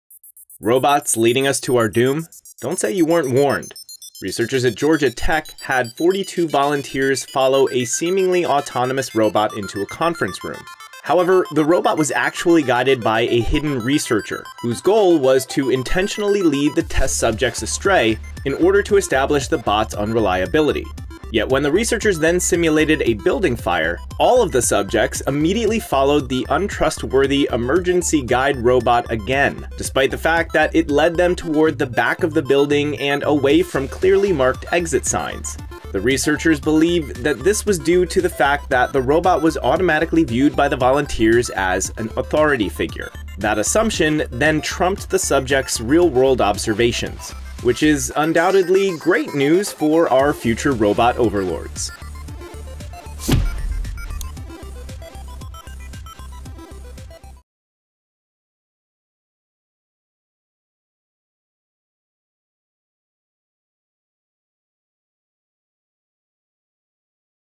Untrustworthy Robots
Researchers at Georgia Tech had 42 volunteers follow a seemingly autonomous robot into a conference room. However, the robot was actually guided by a hidden researcher, whose goal was to intentionally lead the test subjects astray in order to establish the bot’s unreliability.

Yet when the researchers then simulated a building fire, all of the subjects immediately followed the untrustworthy Emergency Guide Robot again, despite the fact that it led them toward the back of the building and away from clearly marked exit signs. The researchers believe that this was due to the fact that the robot was automatically viewed by the volunteers as an “authority figure.”